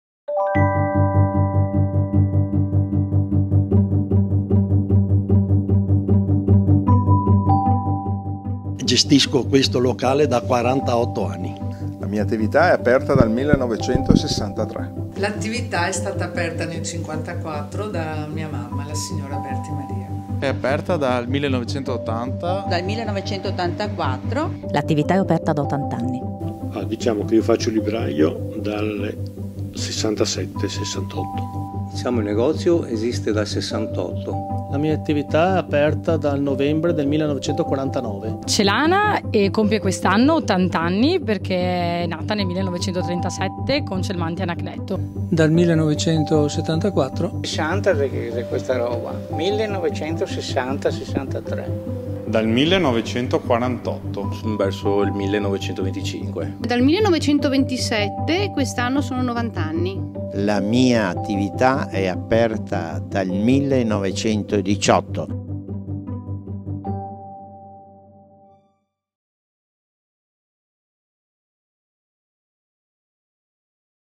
Gli anni delle attività di "Nato e Cresciuto" (teaser)
Teaser della mostra fotografica "Nato e Cresciuto" concepita e realizzata da Holic in collaborazione con il "Comune di Venezia".